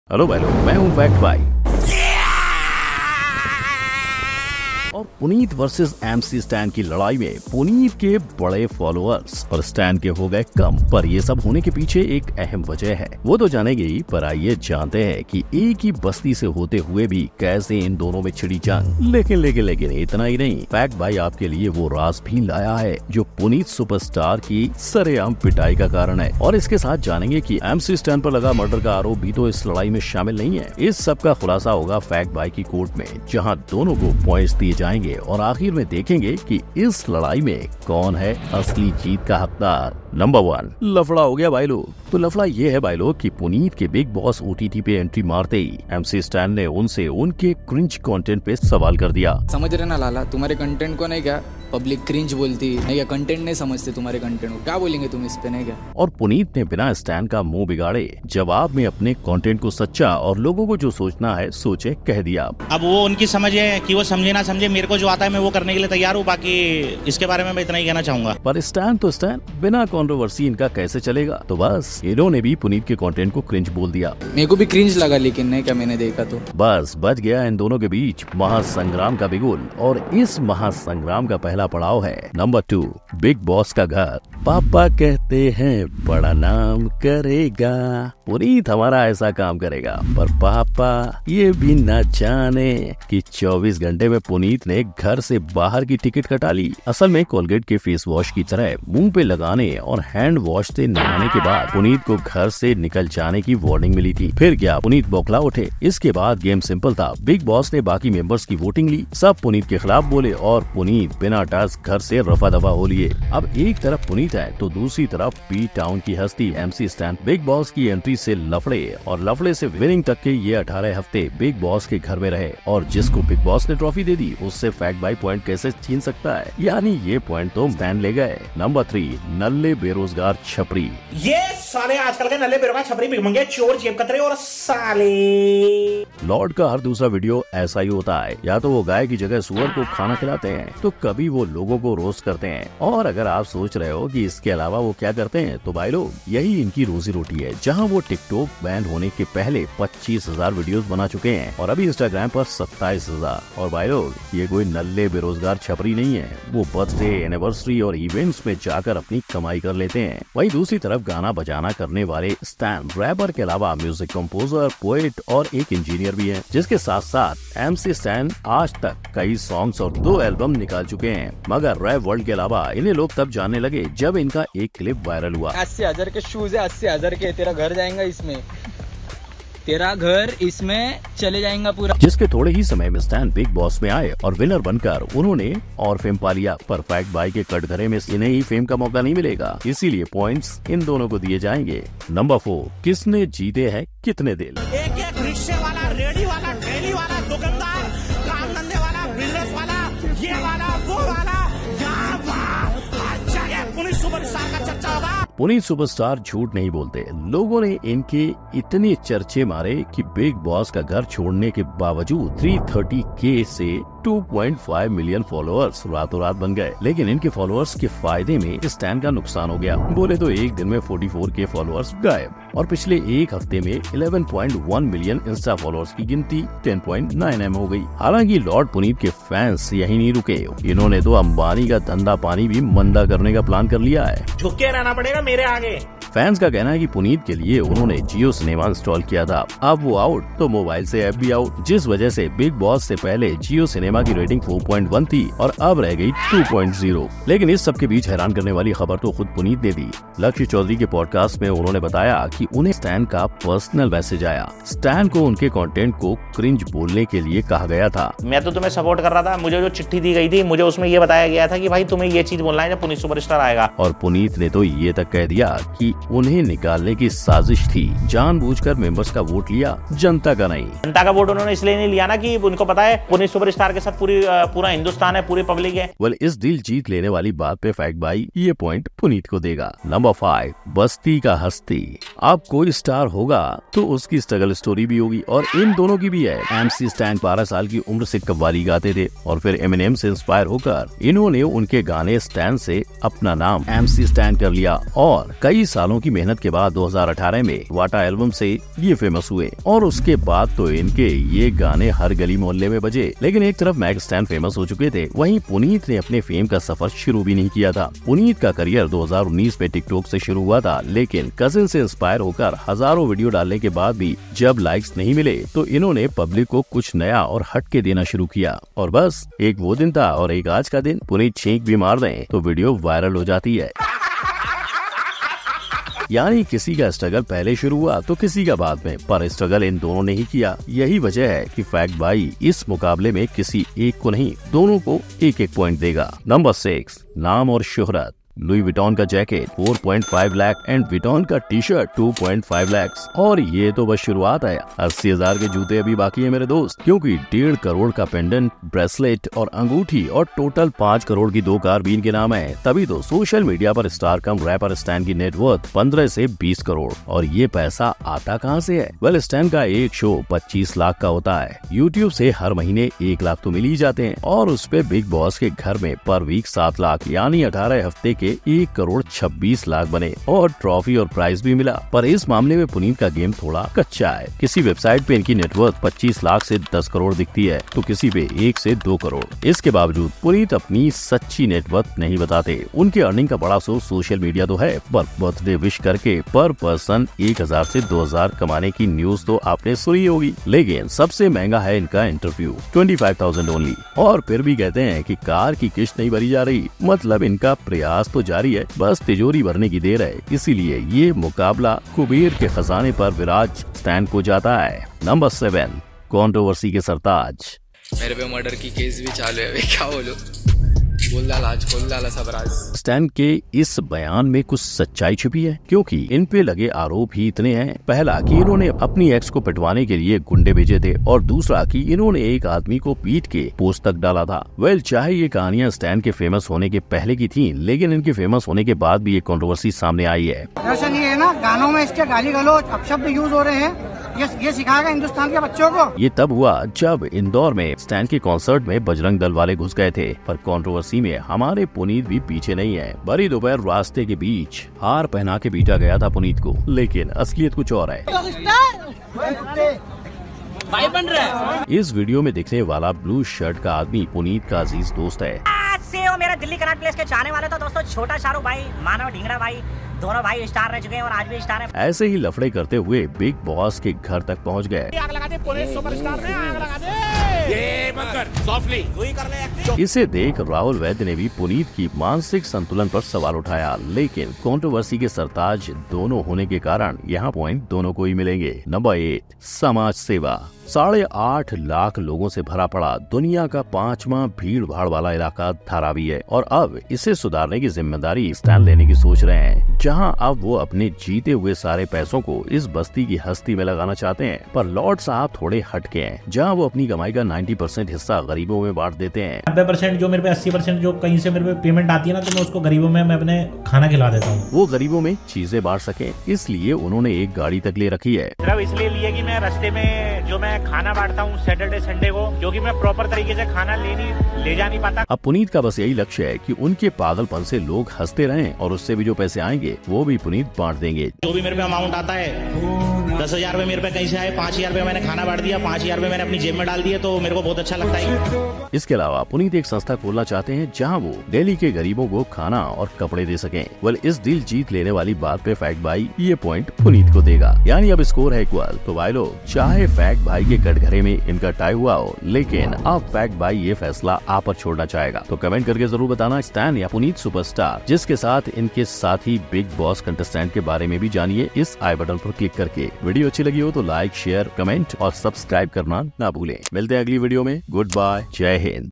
आखिर क्या हुआ Puneet Superstar और MC Stan के बीच | Puneet Superstar VS MC Stan Big Fight In Bigboss
Hello friends, Factbhai is here to tell you all about Puneet Superstar and MC Stan’s fight. This has many angles, what they do, all the controversies, who they rose up to the fame and many more. Today, factbhai will give points to both social media fame and at the end you will decide who wins here. Let’s start.